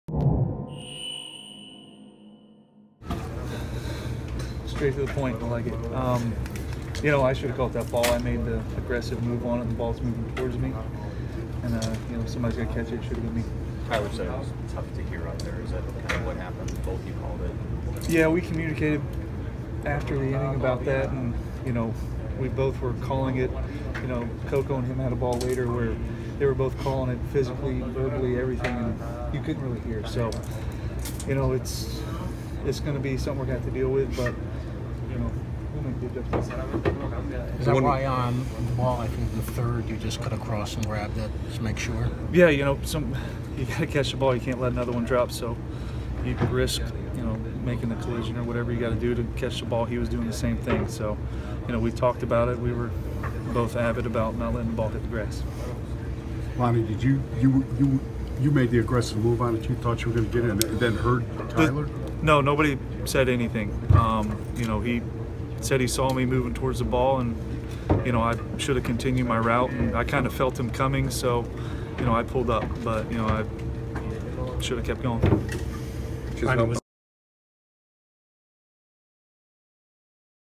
Lonnie Chisenhall didn't call for the ball on misplay in outfield
Indians outfielder Lonnie Chisenhall admitted that he nor Tyler Naquin called for the ball that landed between them in the first inning of Game 6 of the World Series.